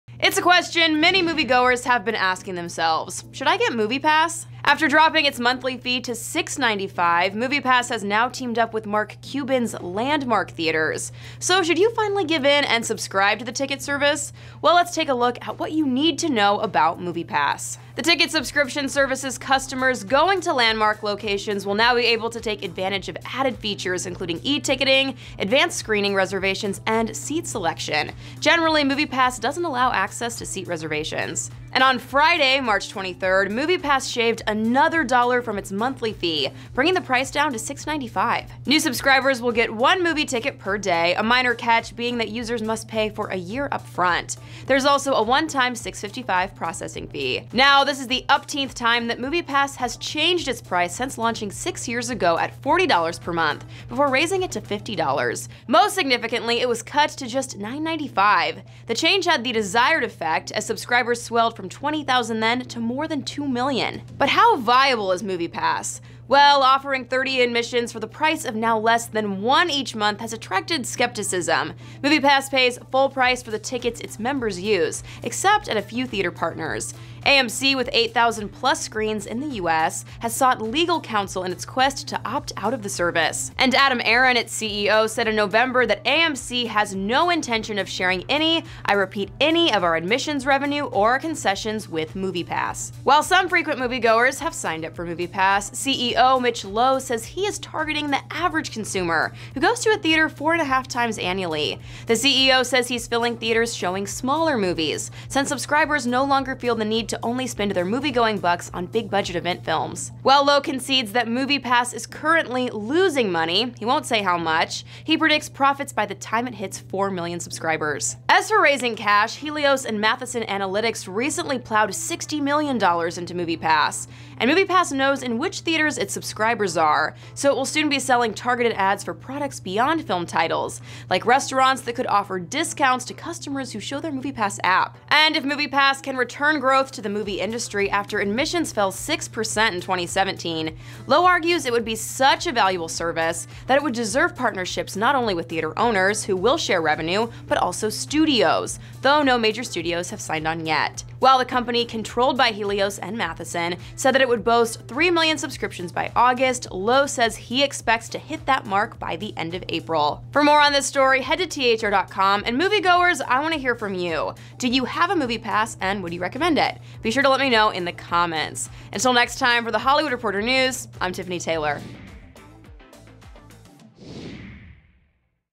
What You Need to Know About MoviePass (So Far) | THR News
"We are excited to offer MoviePass customers access to our Landmark Theatres," said Cuban.